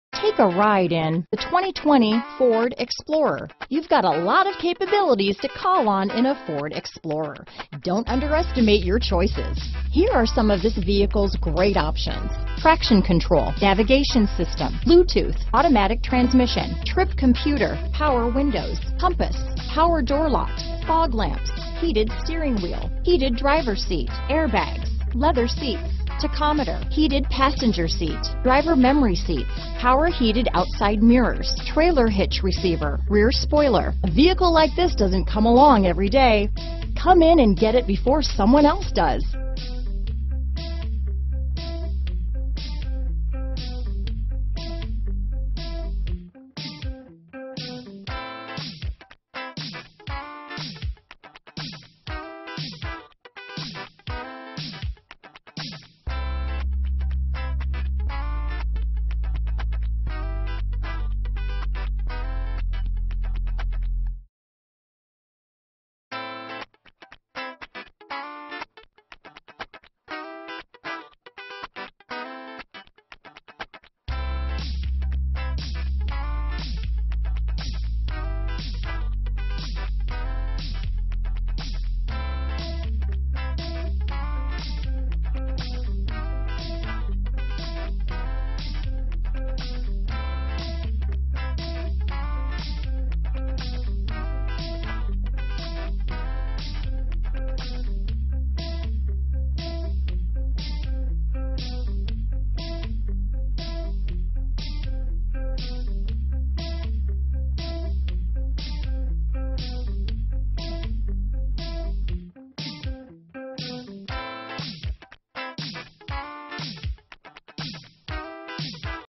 2020 Ford Explorer Sandwich, Oswego, Yorkville, Plano, IL 49040
New 2020 Ford Explorer available in Sandwich, Illinois at Gjovik Ford. Servicing the Oswego, Yorkville, PLano, IL area.

Gjovik Ford
2600 US Rt 34 E